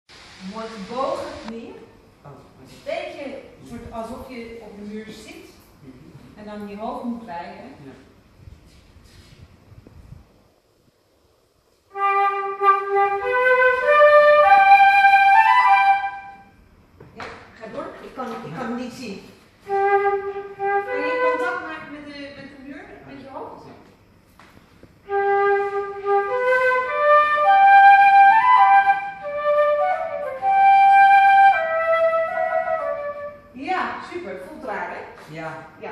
VLOG 6 - 22 juli 2019 - posture practice
Flute players tend to bend over and stretch their neck like a turtle. Back and head against a wall is a good practice method for standing straight. Don't forget to bend your knees.